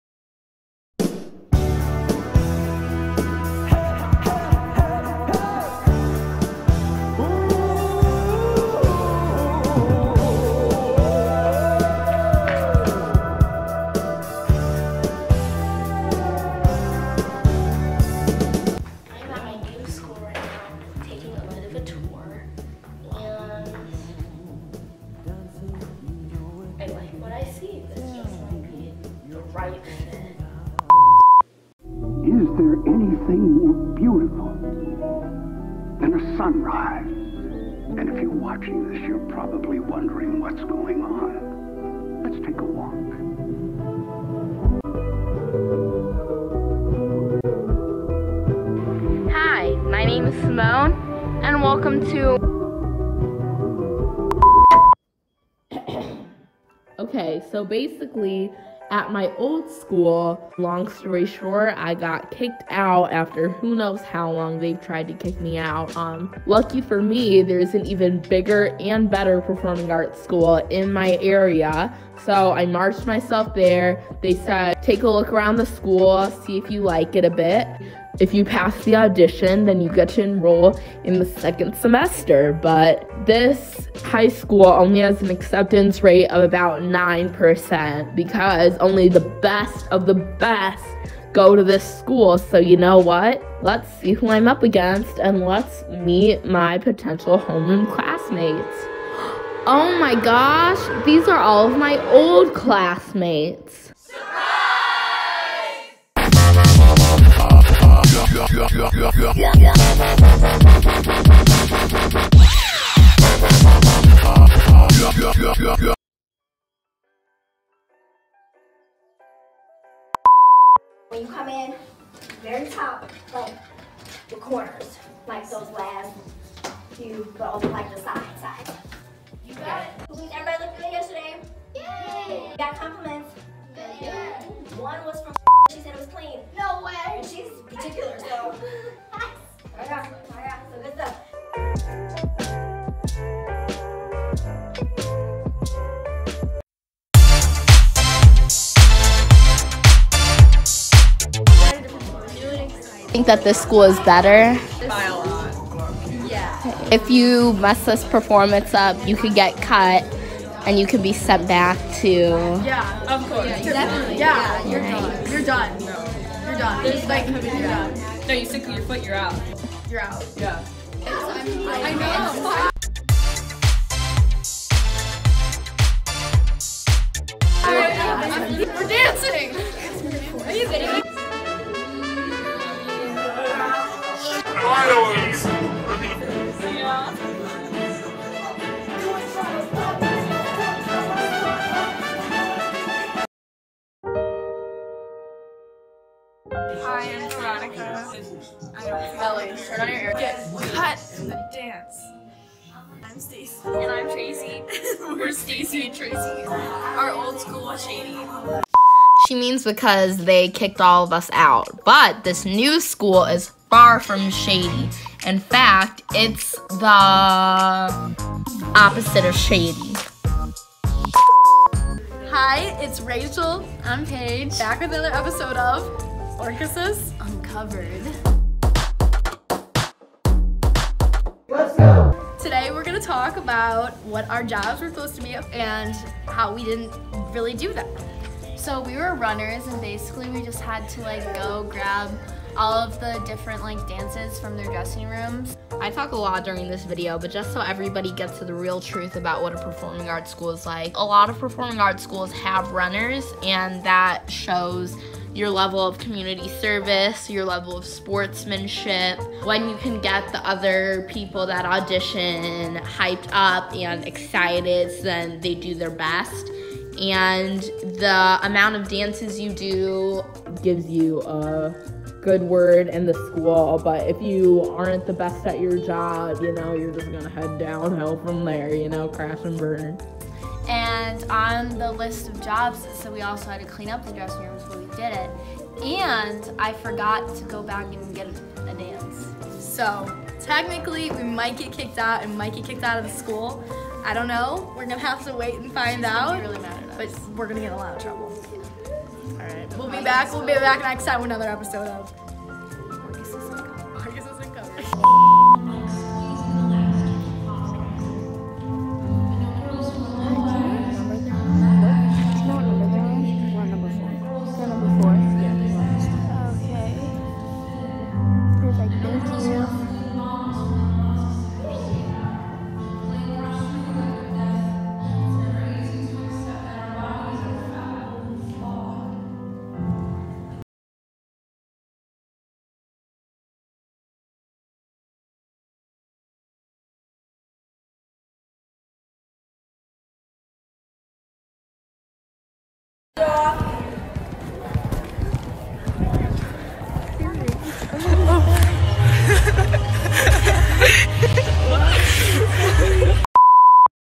THE TRUTH ABOUT MY PERFORMING ARTS SCHOOL- Dance program
A lot of this was fake btw, I don't actually go to a performing arts high school ahaha
(Also I was sick, so my voice is a bit nasally)